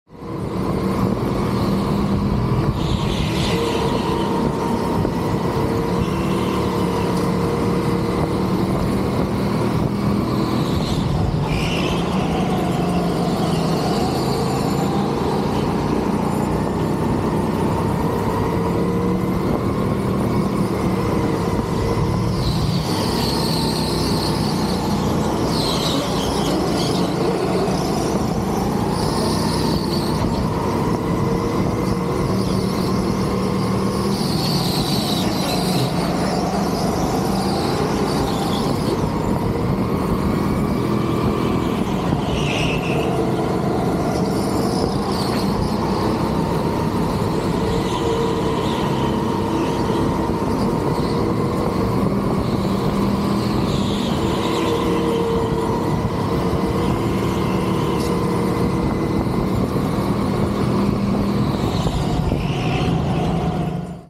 E1GOKART Gdansk (NEW LAYOUT) - Weight 90kg, Best lap 50.873
My best lap at E1Gokart Gdansk - 50.873.